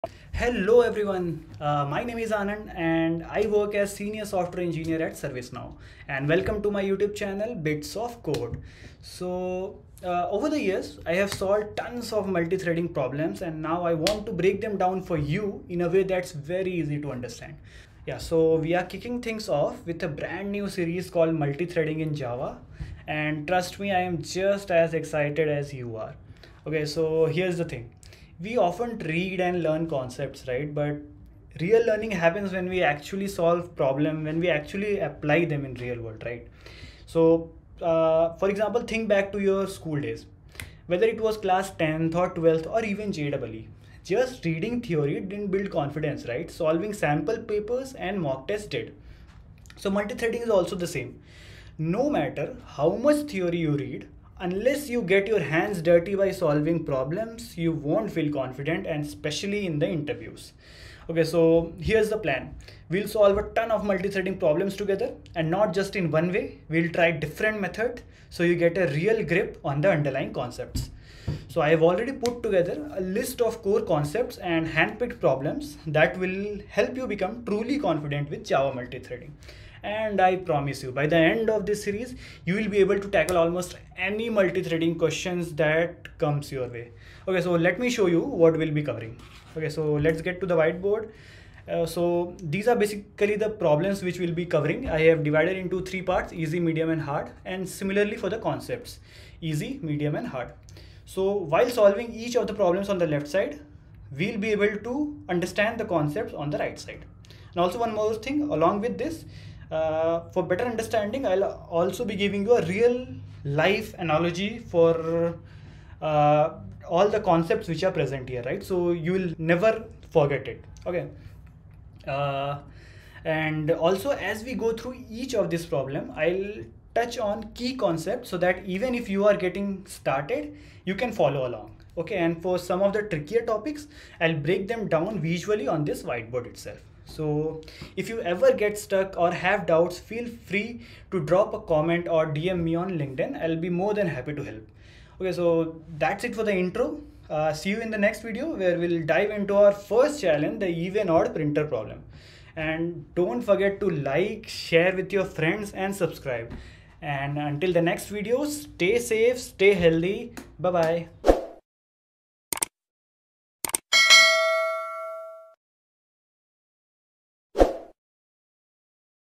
Java Multithreading Series Begins 🚀 | Learn Step-by-Step | Learn by Solving Problems
🚀 Welcome to *Bits Of Code by Anand*!
In this 3-minute video, I’m kicking off an exciting new series: **Multithreading in Java**.

🧵 What to Expect:
- Real-world multithreading problems
- Concepts explained with visuals and examples
- Code walkthroughs & best practices
- Interview-style questions and solutions

Whether you're preparing for interviews or just want to understand how threads work under the hood, this series is for you!